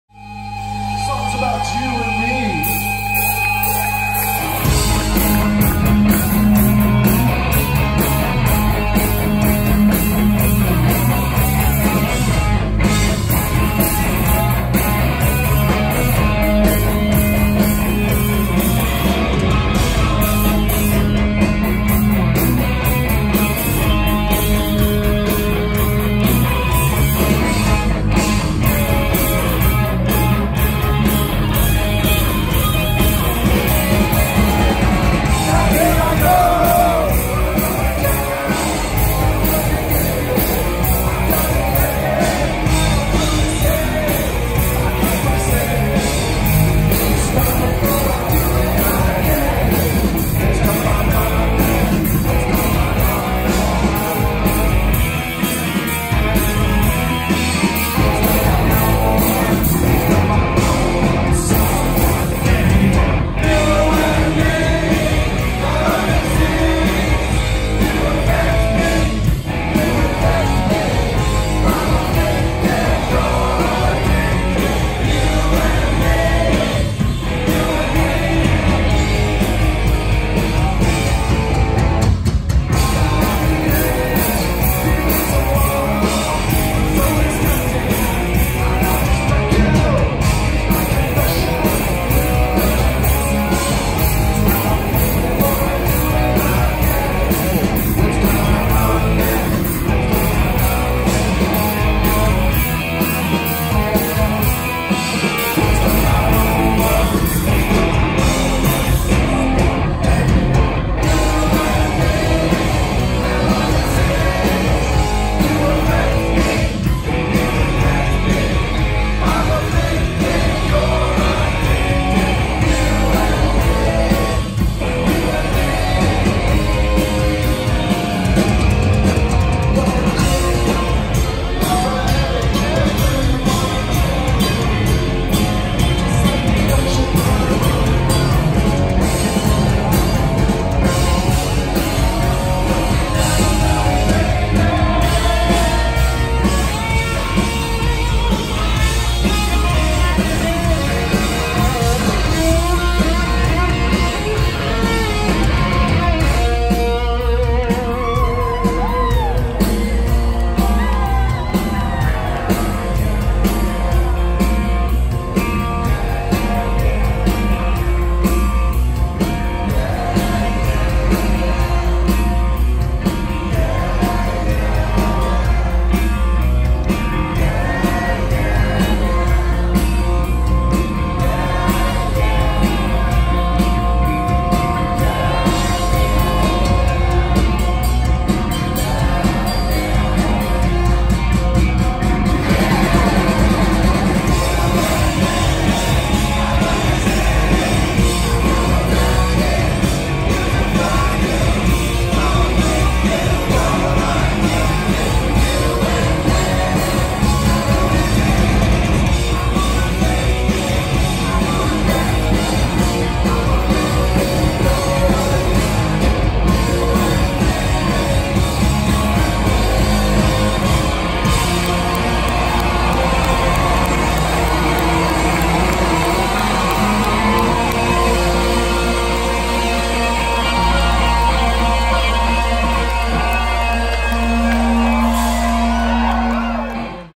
Bad Religion @ The Ogden Theater 7.9.19 no.17
Bad Religion performing live at The Ogden Theater in Denver, Colorado 7.9.19.